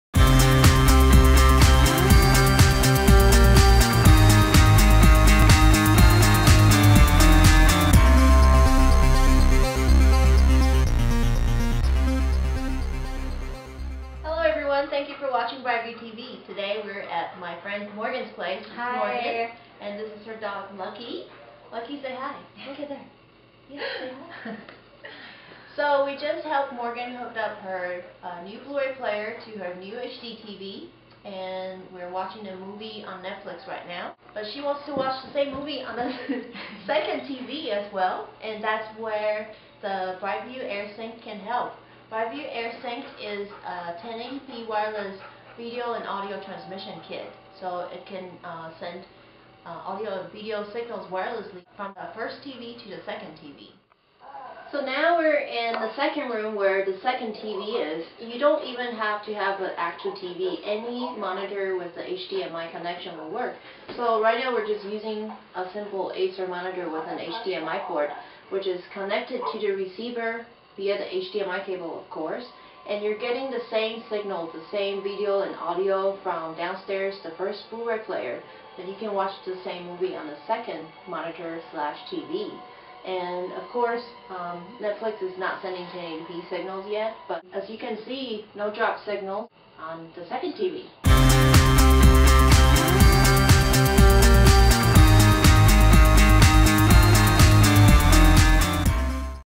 brite-View TV Episode 7: Air SyncHD Sends Audio/Video Signals Through Brick Walls
Check out the Air SyncHD (BV-2322) in a brick house. It beams the Netflix movie (instant streaming) from a blu-ray player attached at the 1st floor to another display on the 2nd floor (non line-of-sight).